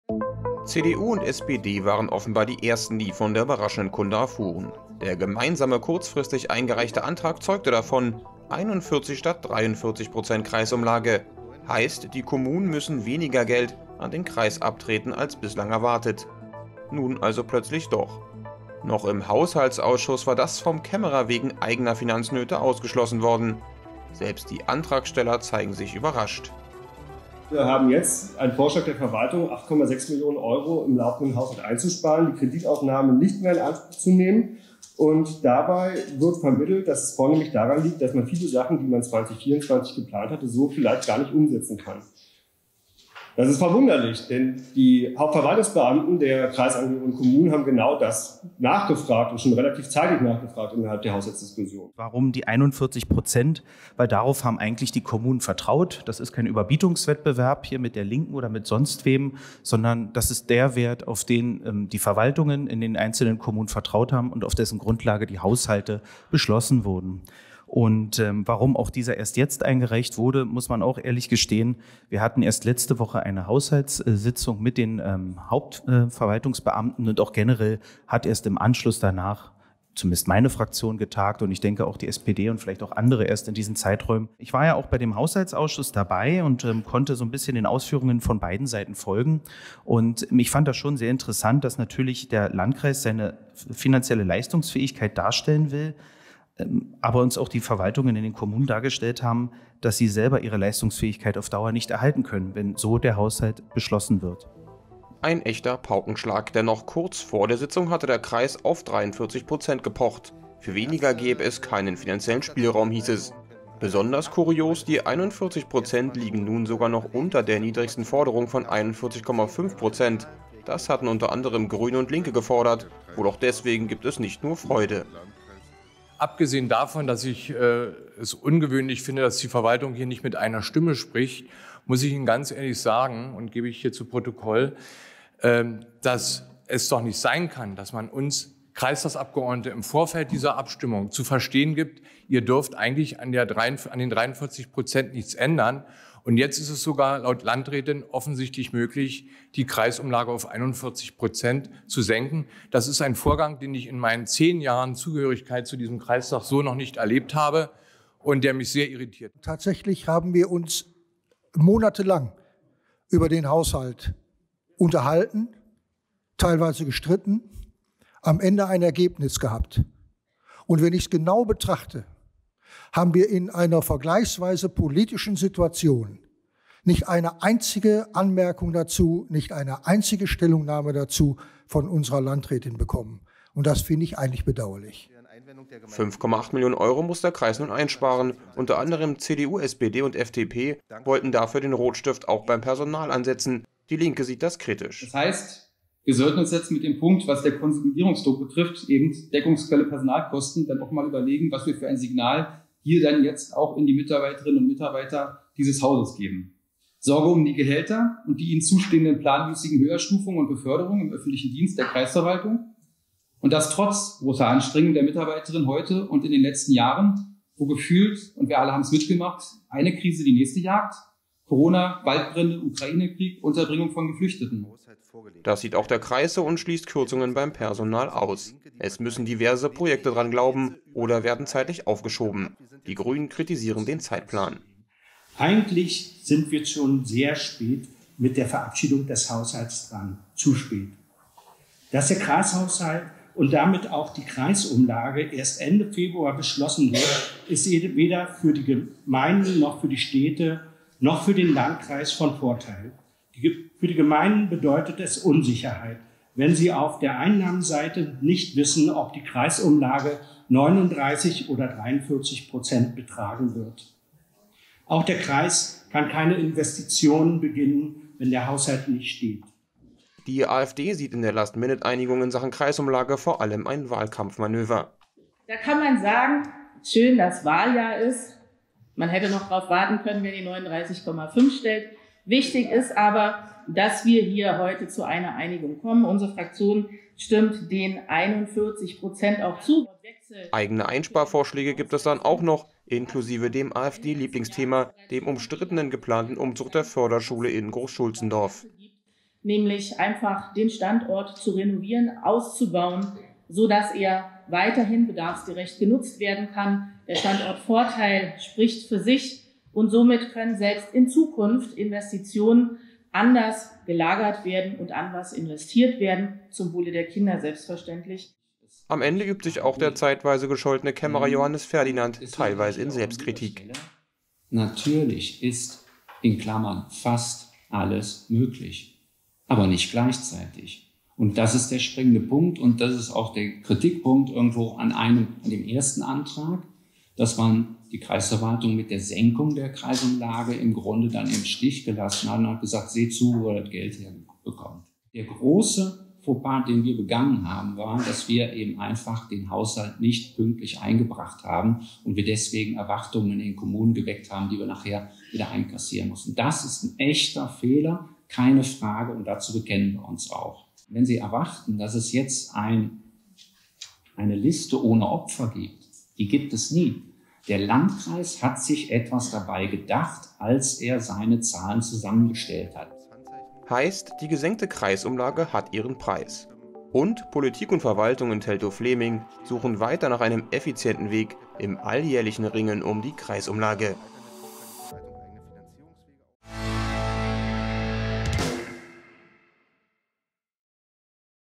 Wende bei der Kreisumlage | Kreistag Teltow-Fläming
Die hitzige Debatte im Kreistag Teltow-Fläming schien beschlossene Sache, doch unerwartet wurde es nur ein laues Lüftchen. Der obligatorische Poker um die Höhe der Kreisumlage in Teltow-Fläming fand unlängst ein überraschendes Ende. Doch obwohl die Kreisverwaltung mit 41% klein beigab, waren nicht alle Kreistagsabgeordnete zufrieden.